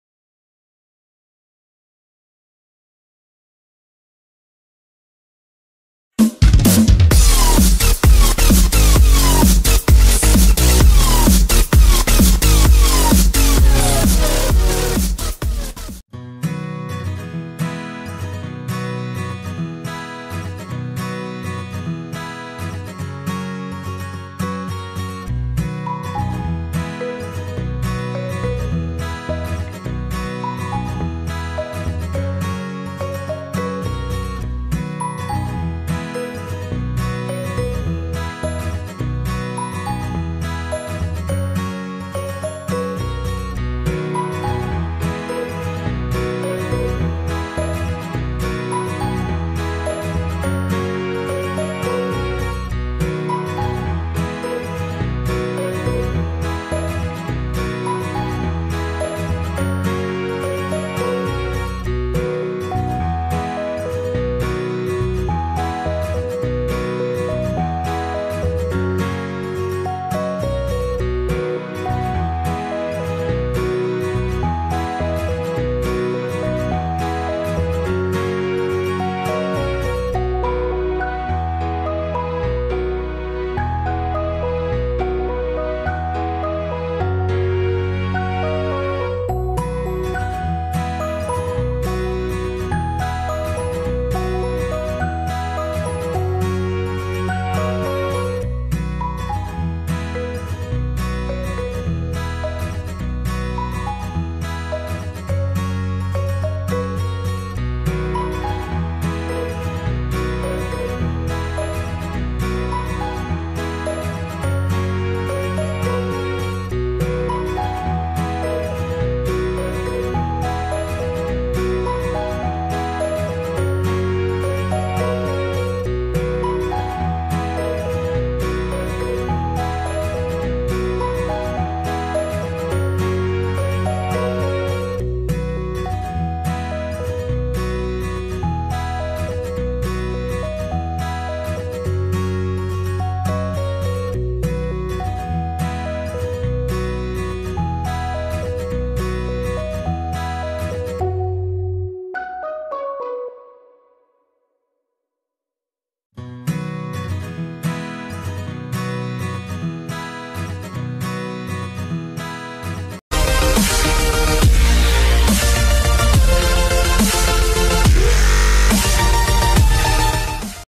"WaPo Bodensee" löst "Dr. Kleist" am Vorabend ab
Trotz mäßiger Quoten hat sich die ARD bekanntlich dazu entschieden, die Krimiserie "WaPo Bodensee" in die zweite Staffel zu schicken. Diese wird ab Anfang März den Weg ins Programm schaffen und den Sendeplatz von "Familie Dr. Kleist" übernehmen.
Das Erste hat eine Fortsetzung seiner Vorabendserie "W ...